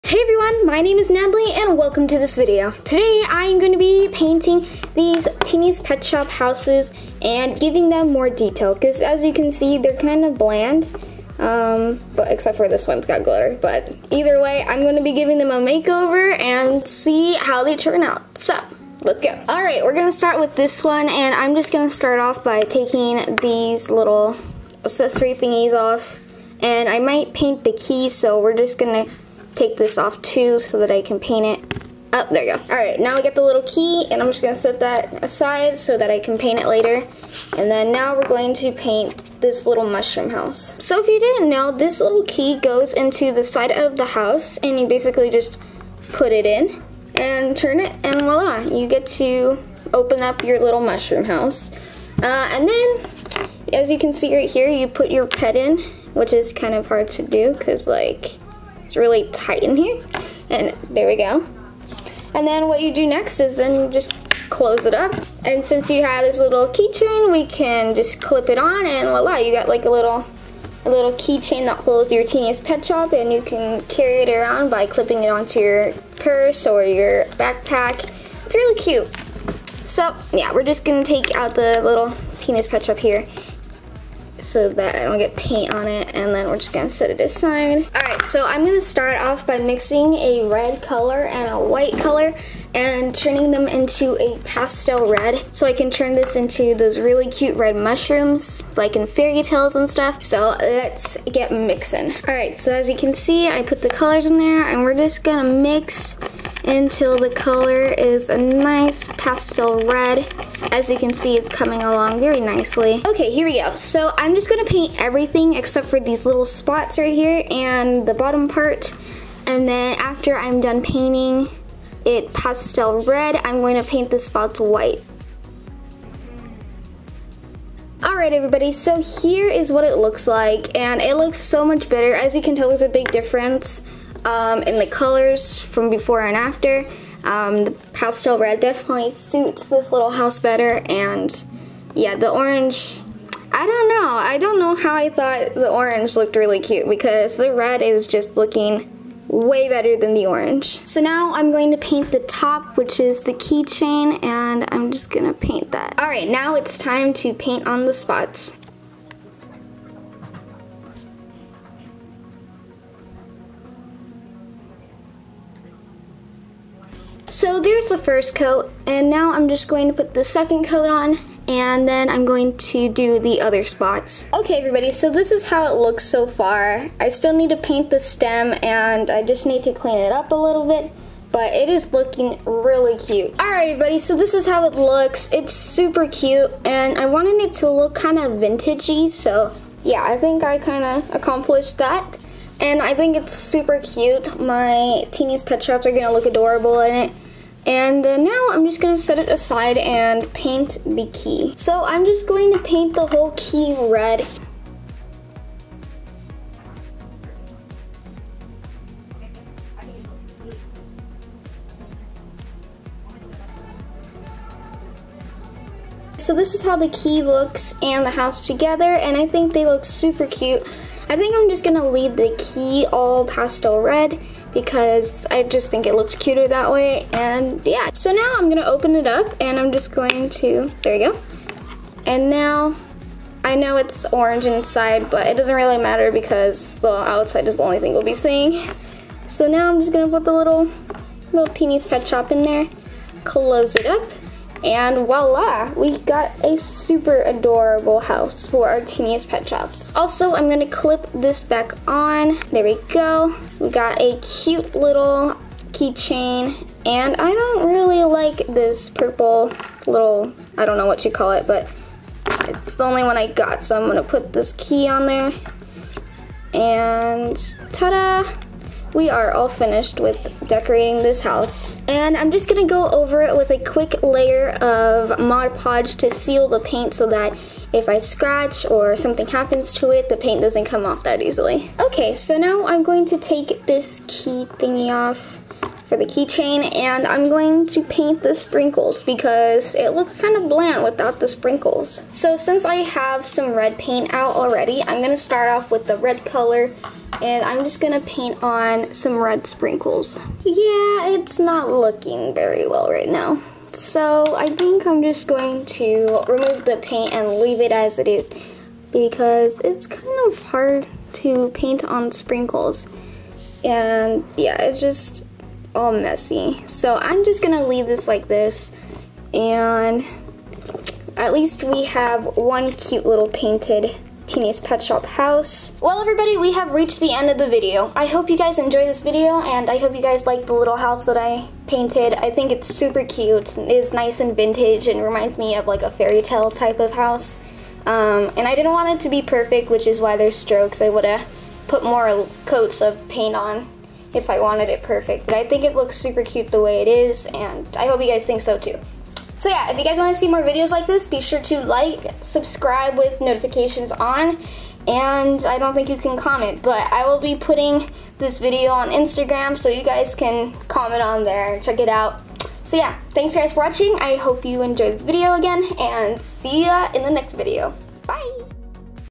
DIY Teeniest Pet Shop House
Hey everyone, still can't get over how cute the house turned out! If you guys end up painting your own post it and tag me on instagram! :)